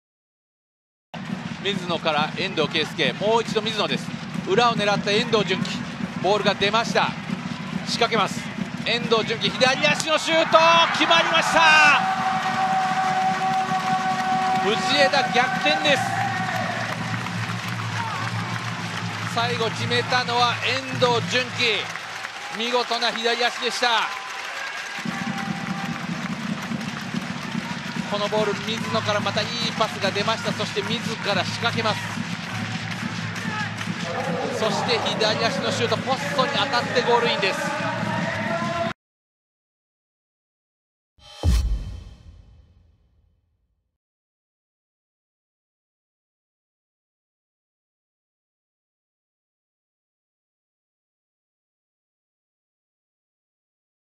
【公式】ゴール動画：遠藤 純輝（藤枝）45分 藤枝ＭＹＦＣvsガンバ大阪Ｕ－２３ 明治安田生命Ｊ３リーグ 第27節 2018/10/13
スルーパスに抜け出した遠藤 純輝（藤枝）がドリブルでPA内に侵入すると、低い弾道のシュートがポストを叩きながらもゴールに吸い込まれる！明治安田Ｊ３のゴール動画を公開中！
2018年10月13日（土）に行われた明治安田生命Ｊ３リーグ 第27節 藤枝vsＧ大23のゴール動画です。